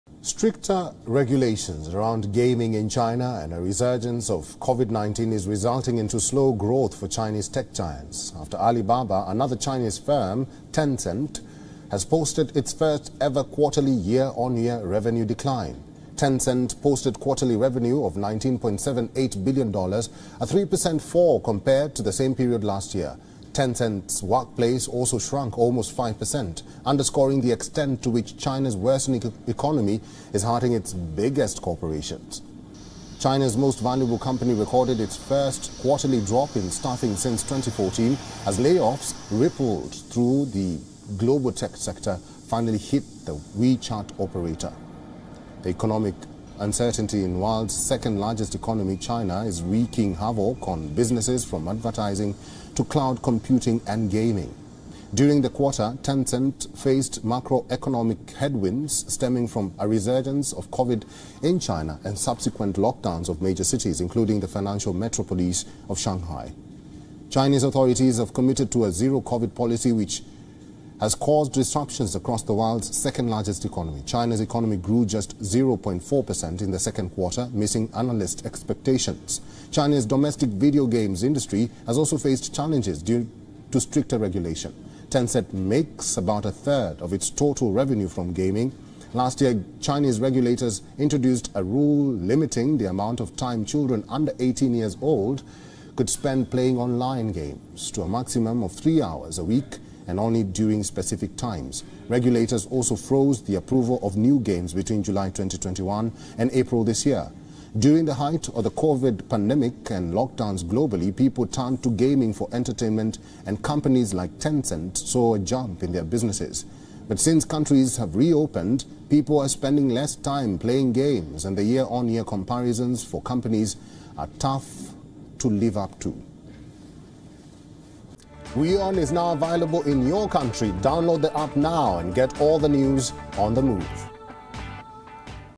WION Dispatch: Tencent posts first revenue decline since going public | China | Latest English News
Tencent, China’s largest video gaming and social media company, reported its first revenue decline since its 2004 listing and missed earnings expectations.